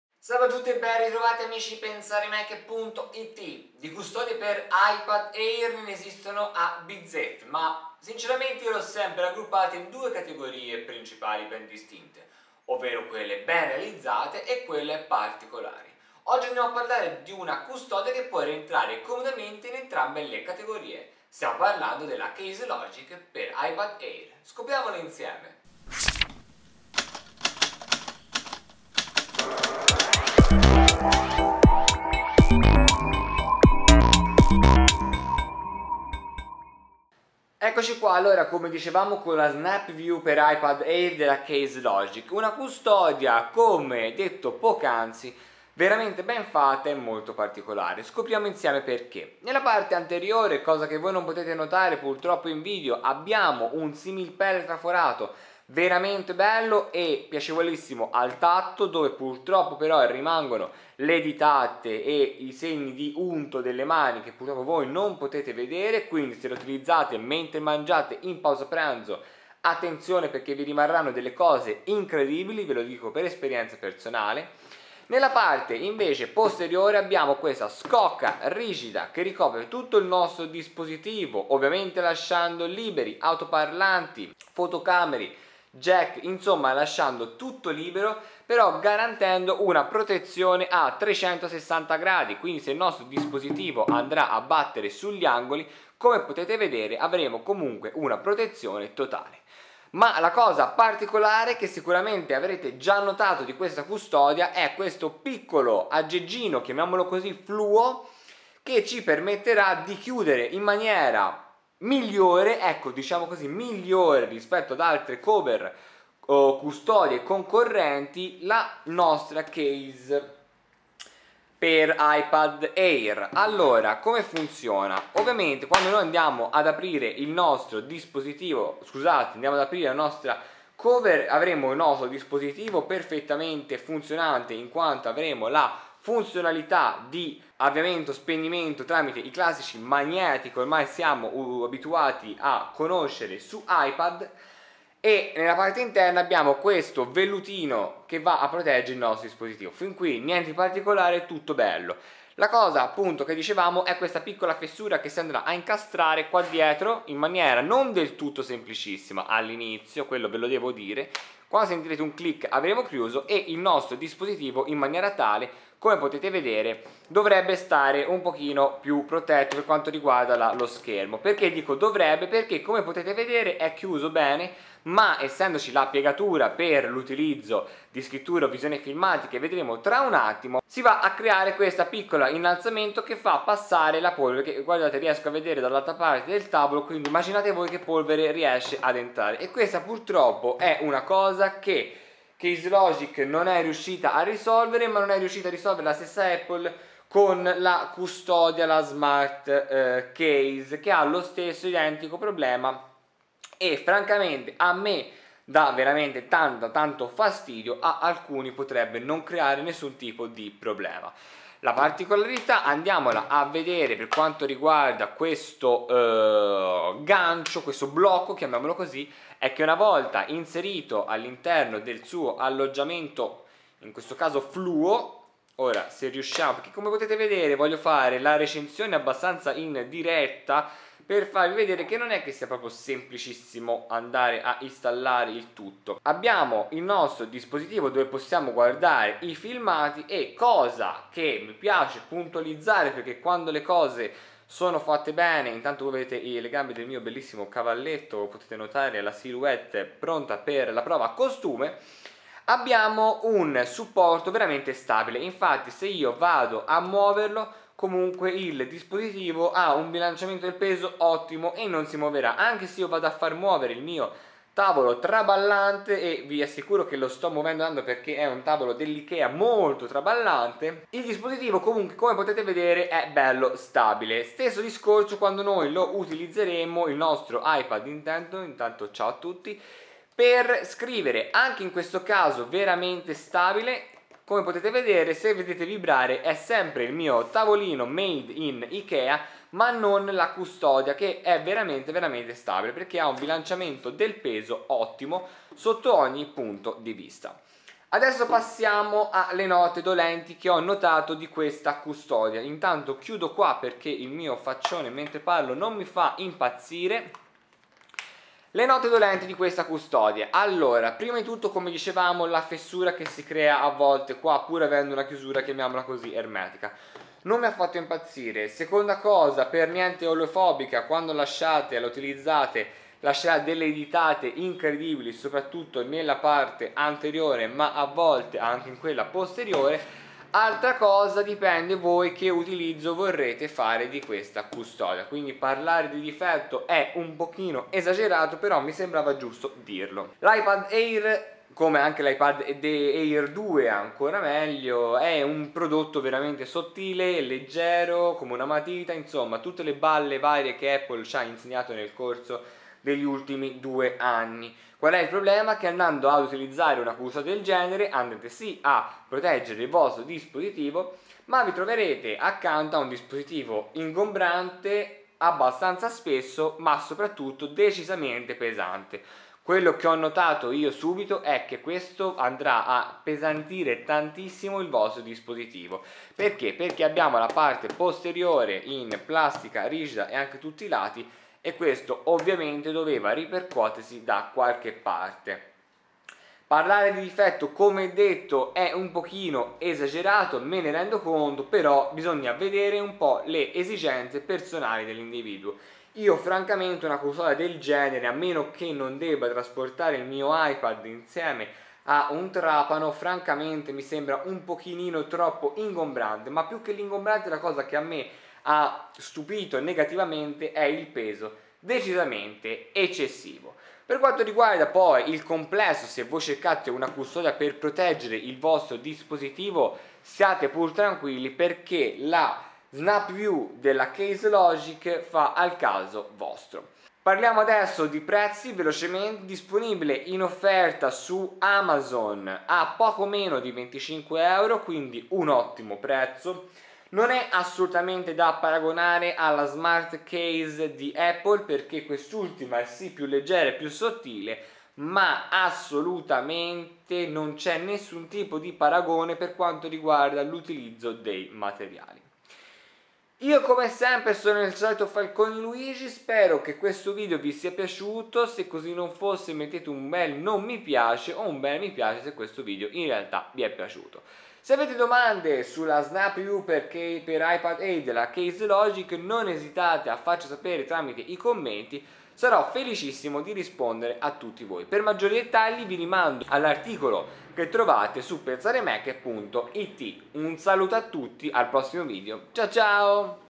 Case Logic snapview per iPad Air
Di custodie per iPad Air ne esistono a bizzeffe, ma questa ha qualcosa di davvero particolare (ed utile); cosa? Scopriamolo insieme!

Acquisto su Amazon